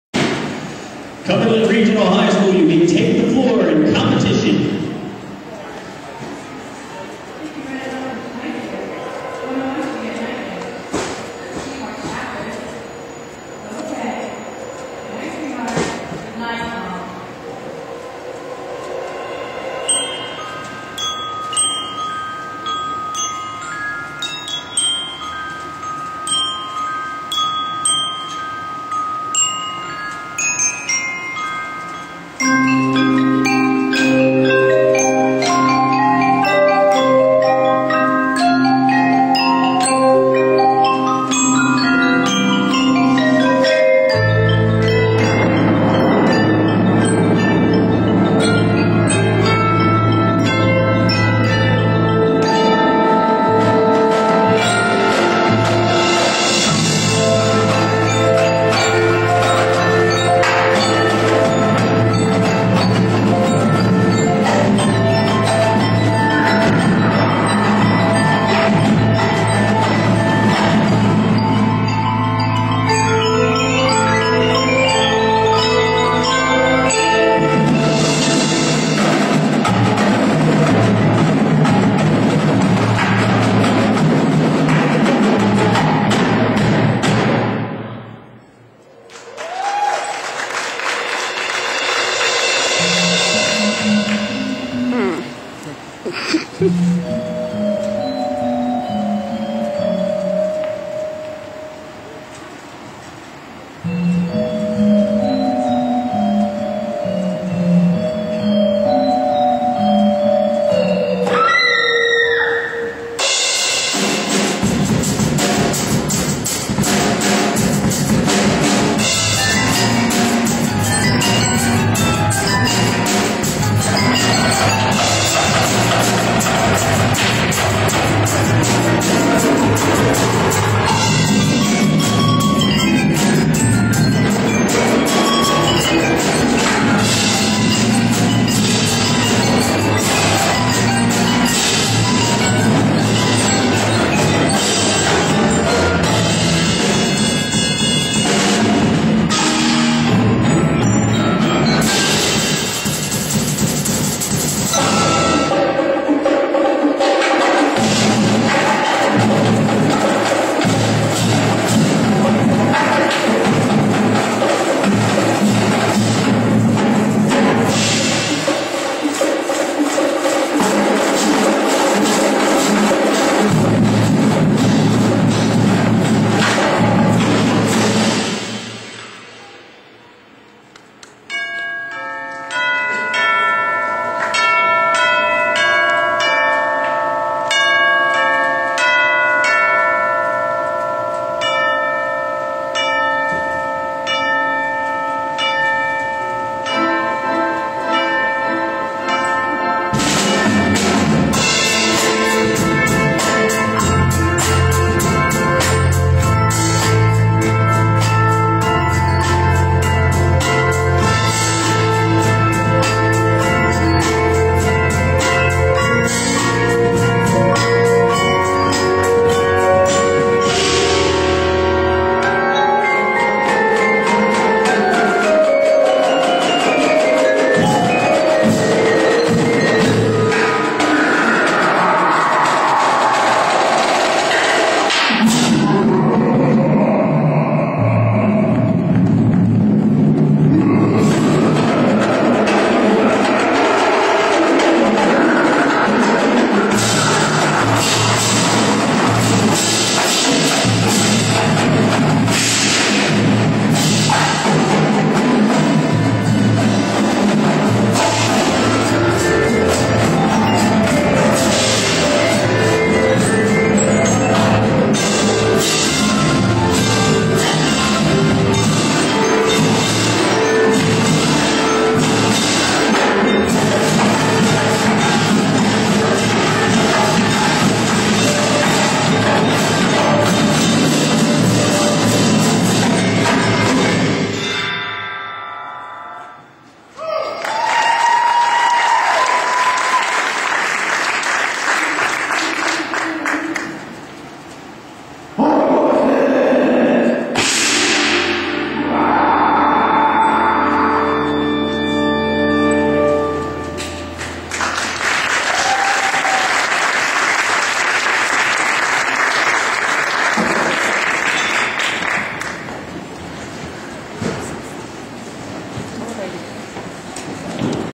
Cumberland Regional Highschool Indoor Percussion: The Book
this is our show called the Book...
i apologize for my mom's video skills... lmao
our performance at Highland Highschool
leave a comment on our performance
we got second place we were down 7 points because of retarded judges